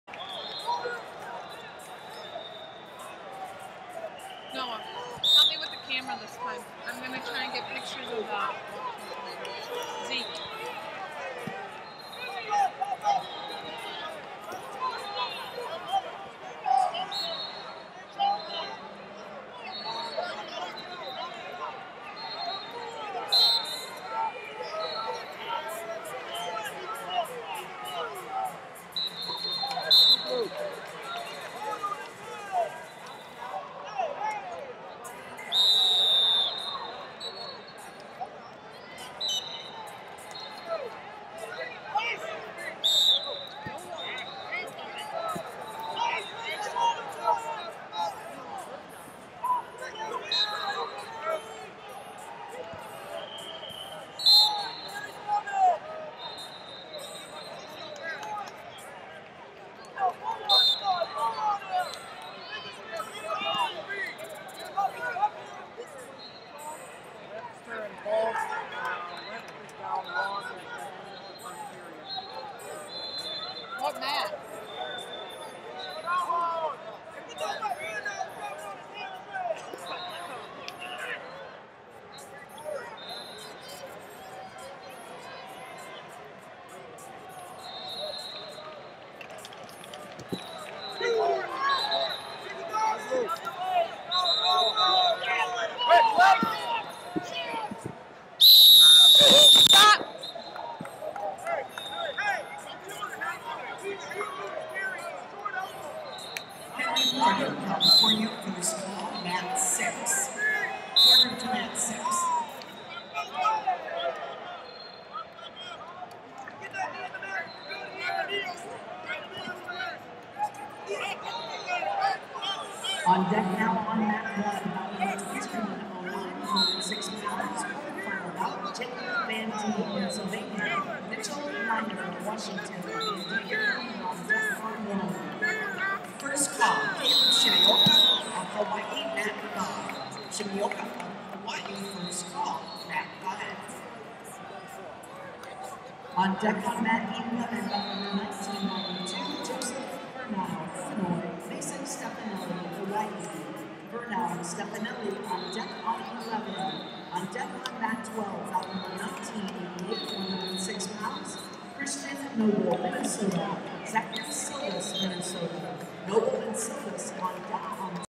132 Lbs Rnd Of 128 Teague Travis Missouri Vs Jospeh Pins Iowa
2019 USMC USAW Junior And Cadet National Championships - 132 Lbs Rnd Of 128 Teague Travis Missouri Vs Jospeh Pins Iowa